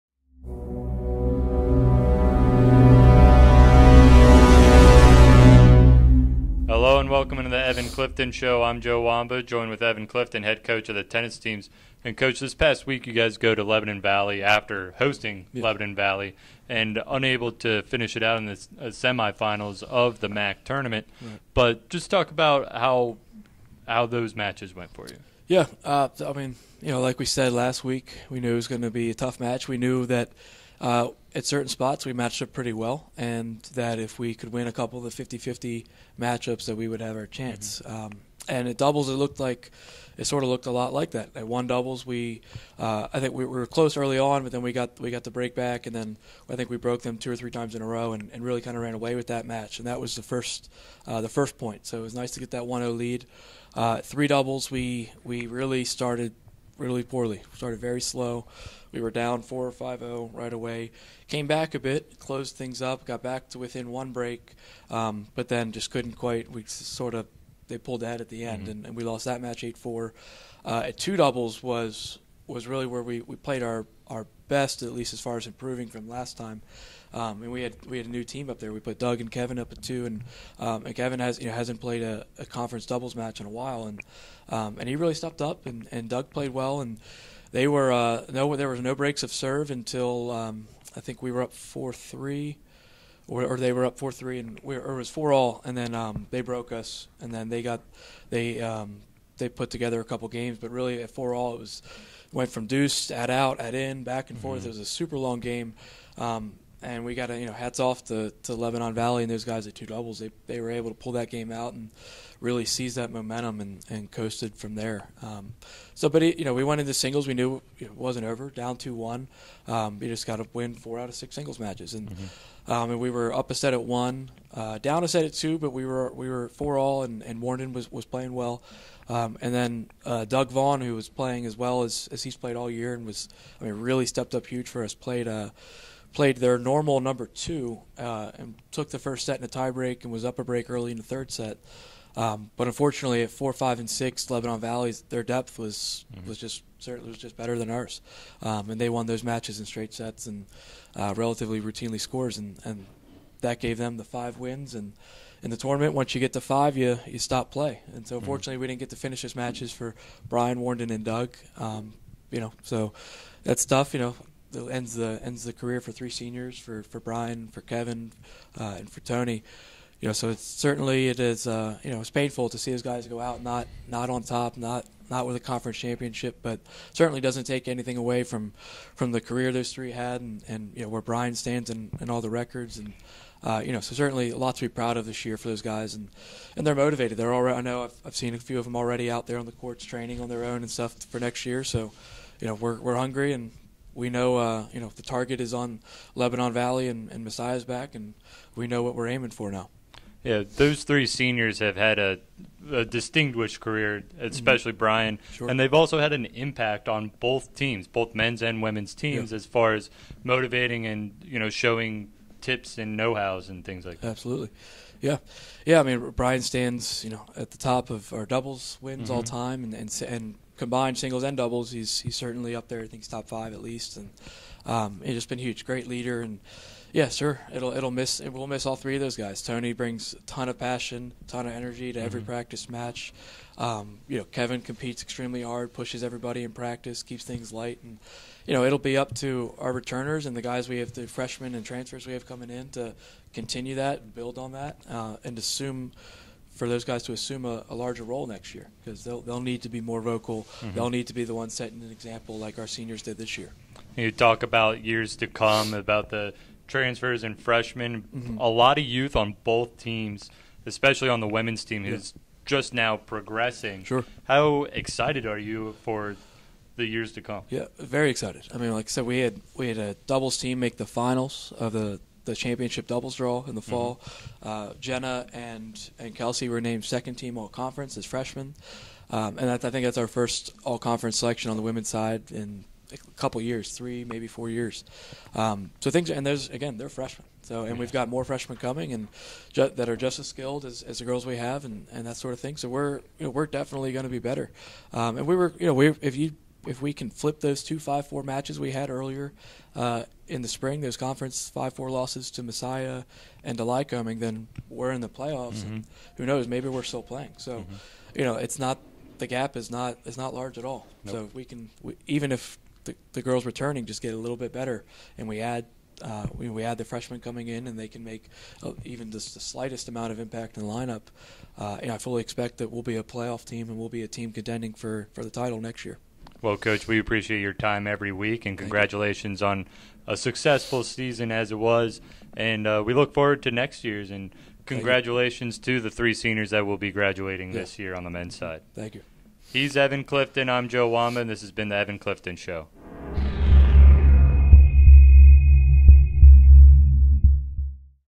The Evan Clifton Show Episode 10
Tennis head coach Evan Clifton talks about the finish to the 2017 season.